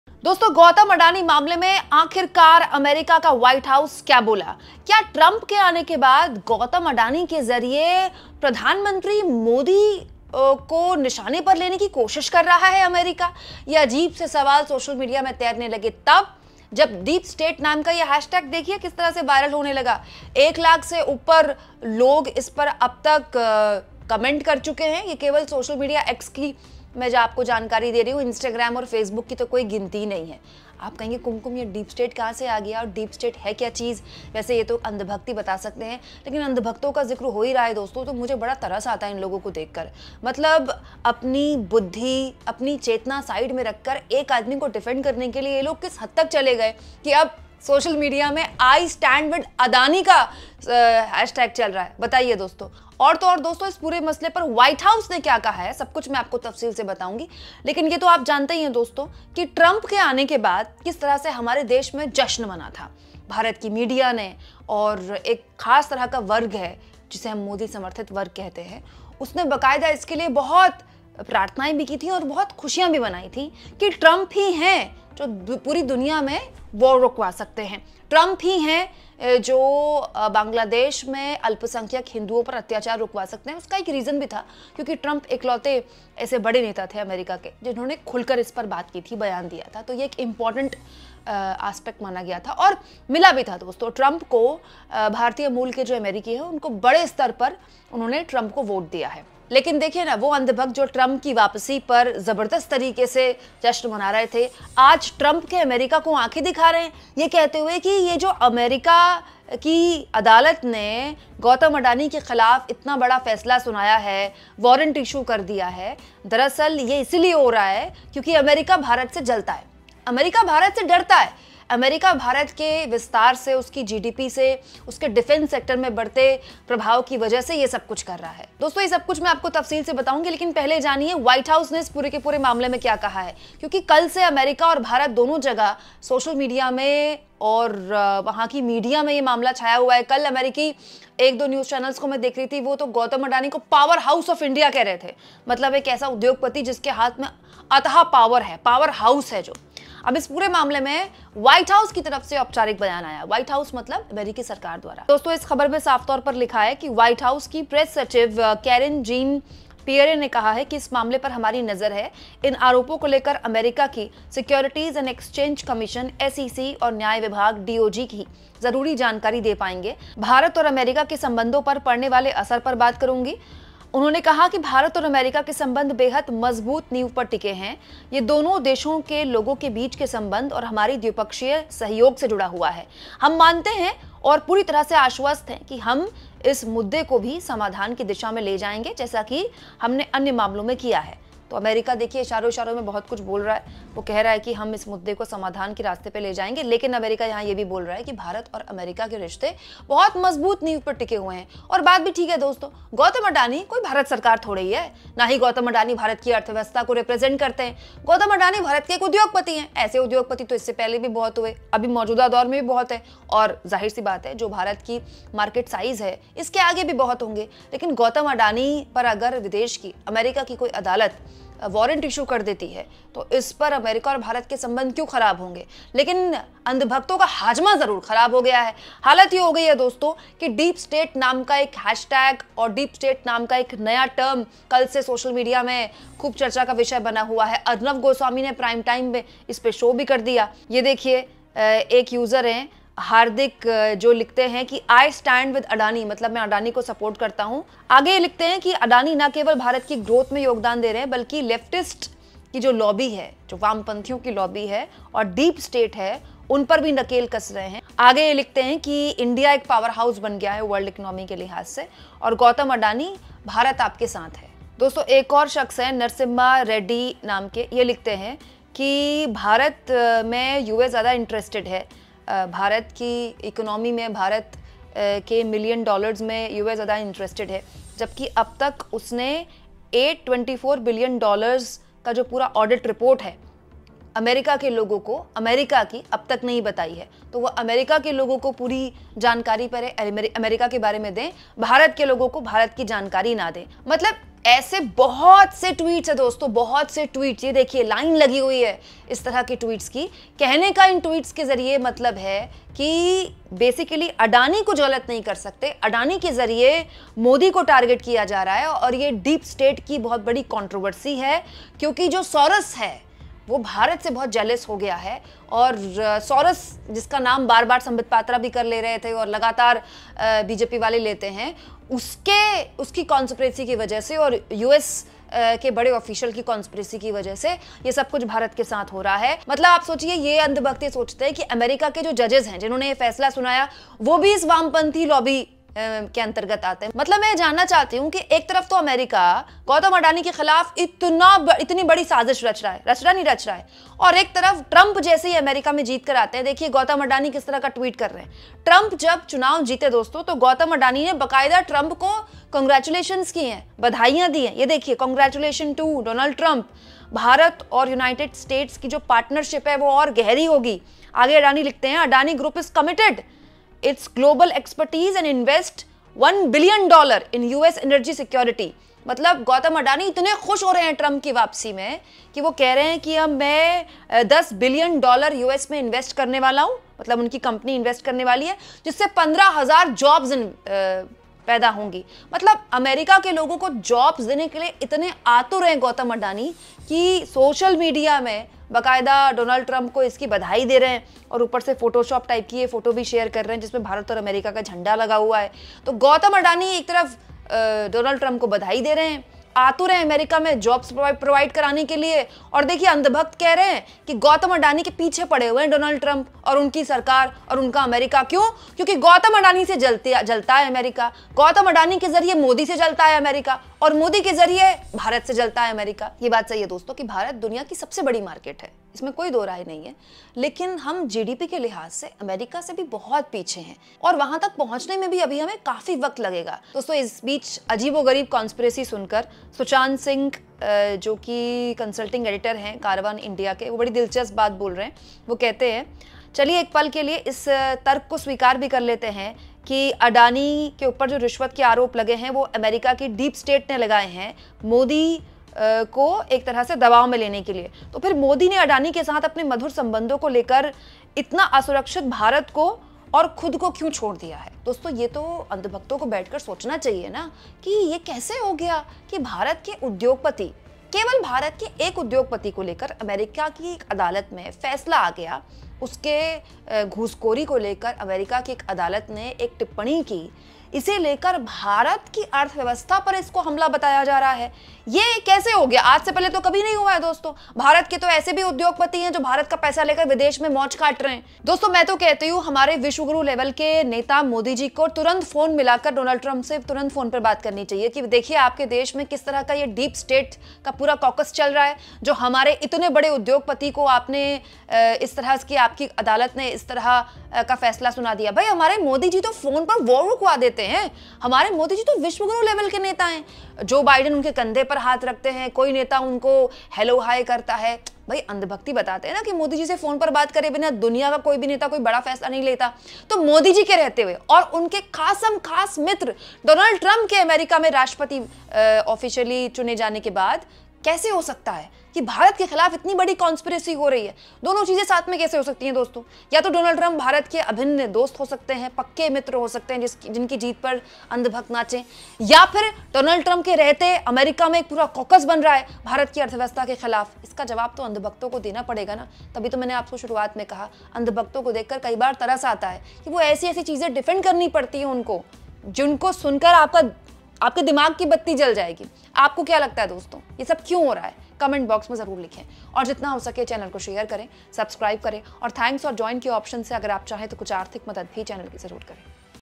Gautam Adani पर अंधभक्त Trump पर भड़के तो! America का white house क्या बोला?
Now the White House's statement has come amidst allegations of veteran businessman Gautam Adani offering bribe to get contracts for solar energy projects. White House Press Secretary Karine Jean Pierre said that we are keeping an eye on this matter. Kumkum Binwal analysis.